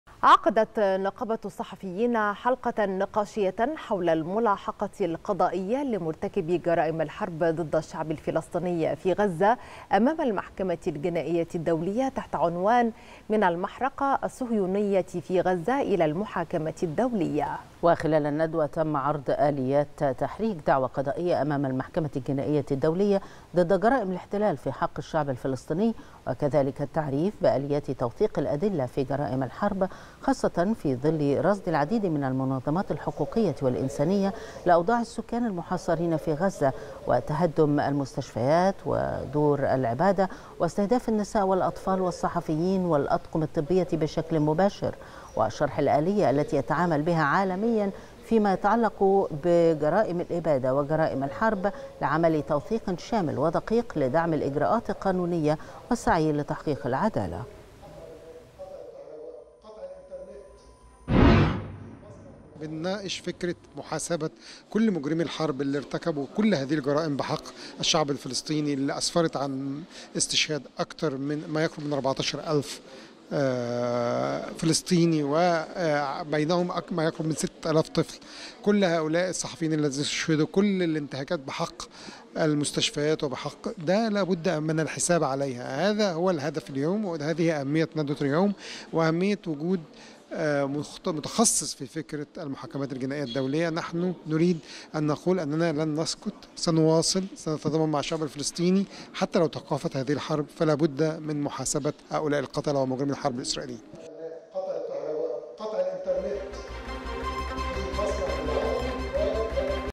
نقابة الصحفيين تعقد ندوة حول الملاحقة القضائية لمرتكبي جرائم الحرب ضد الشعب الفلسطيني
نقابة الصحفيين تعقد ندوة حول الملاحقة القضائية لمرتكبي جرائم الحرب ضد الشعب الفلسطيني أمام المحكمة الجنائية الدولية